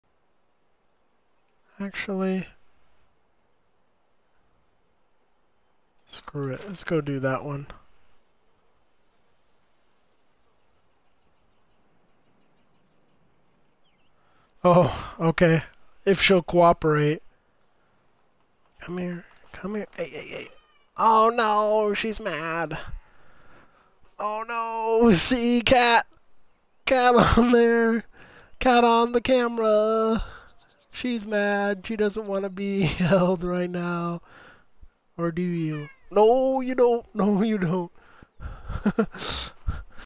Cat Stream!
Whether she wanted to or not, Aurora made her stream debut, lol.